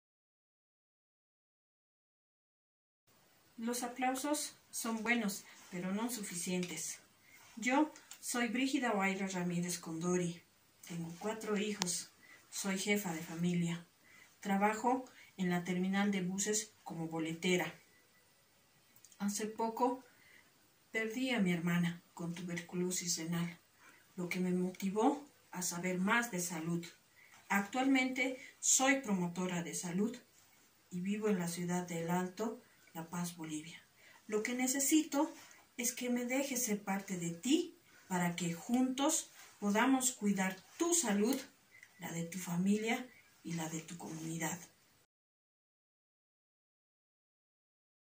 Expanding Healthcare Access in Bolivia
Meet Brigida Wayda Ramirez, one of Pro Mujer's Community Health Workers in Bolivia. Brigida brings preventive health care and education to communities in Bolivia's remote areas.